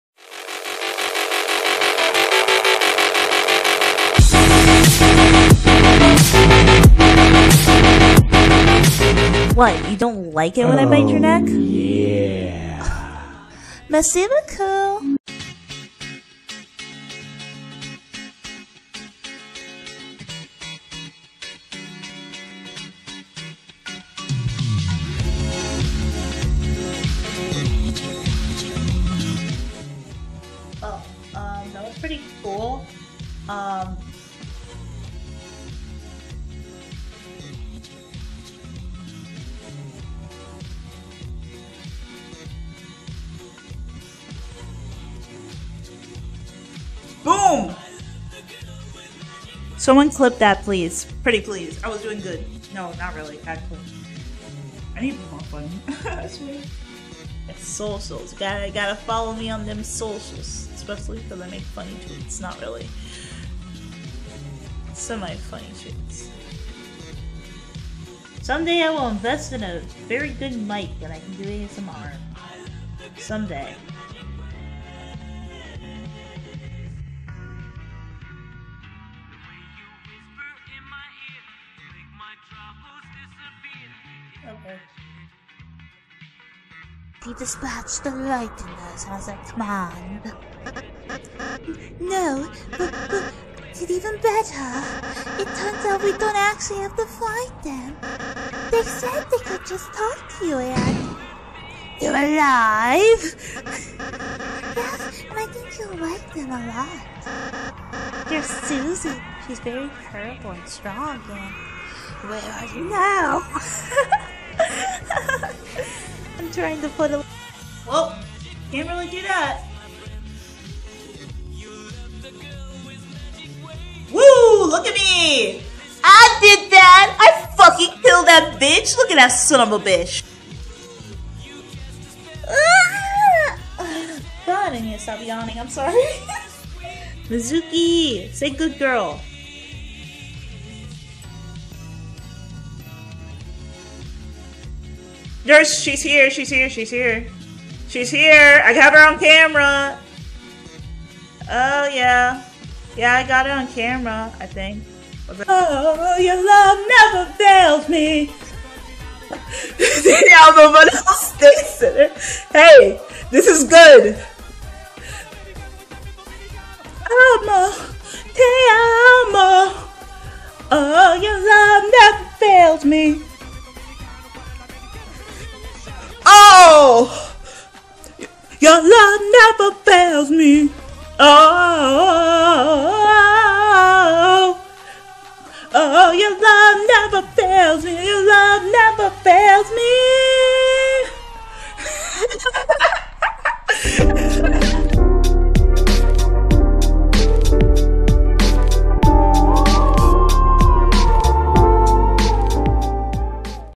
witness CRINGE [Month 1 Highlight]
Witness the highlights of Demi's streams of September & October '21.